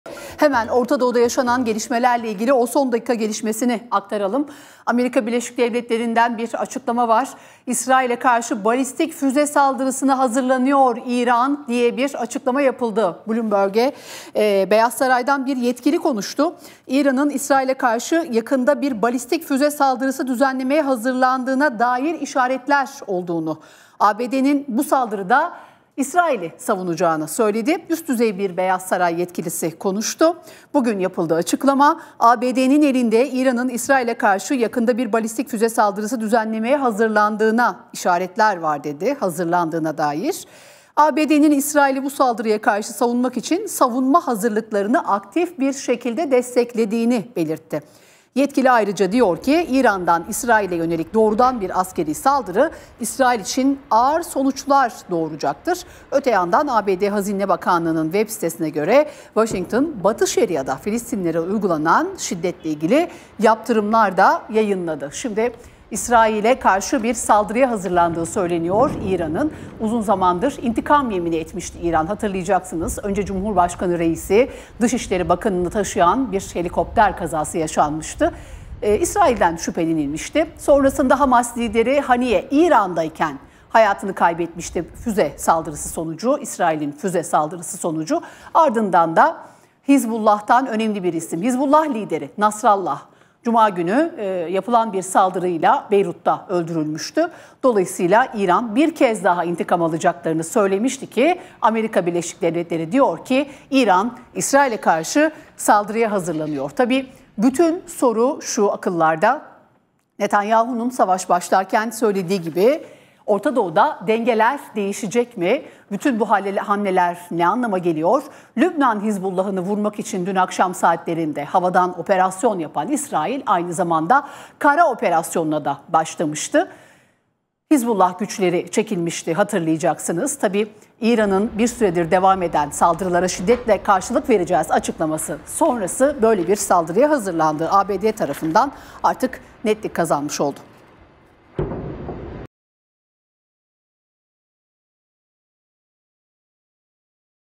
İran'ın İntikam Planını ABD Duyurdu! İsrail'i Füze Yağmuruna Tutacak! Orta Doğu Alev Alacak
Digiturk: 60
D-smart: 93
Turkcell TV Plus: 69
Tivibu: 69
Türksat Kablo TV: 76